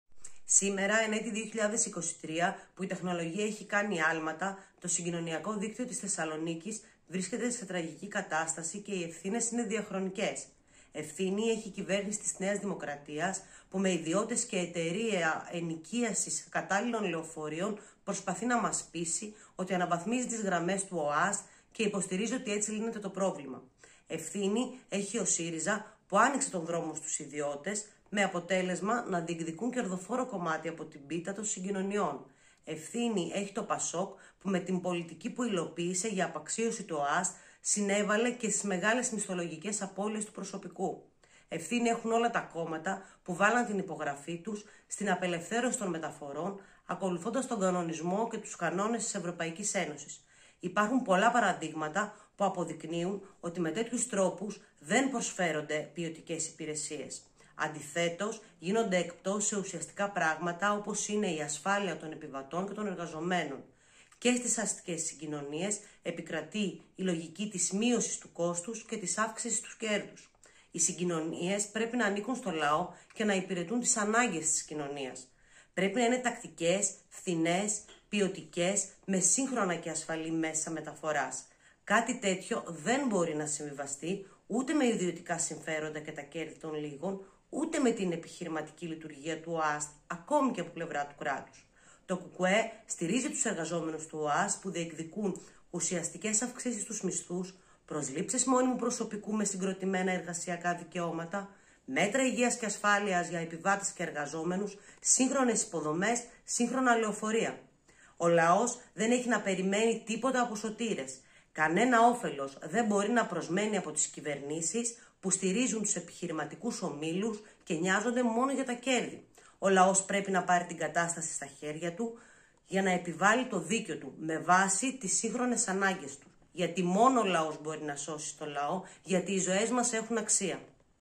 Δήλωση της Αθανασίας Αγοραστού με αφορμή τις κινητοποιήσεις των εργαζόμενων του ΟΑΣΘ
Στήριξη του ΚΚΕ στον αγώνα των εργαζομένων του ΟΑΣΘ

Με δήλωσή της, η Αθανασία (Νάσια) Αγοραστού, υποψήφια Α' Θεσσαλονίκης του ΚΚΕ, με αφορμή τις κινητοποιήσεις των εργαζόμενων του ΟΑΣΘ την Πέμπτη 06/04, εκφράζει τη στήριξη του ΚΚΕ στον αγώνα των εργαζομένων.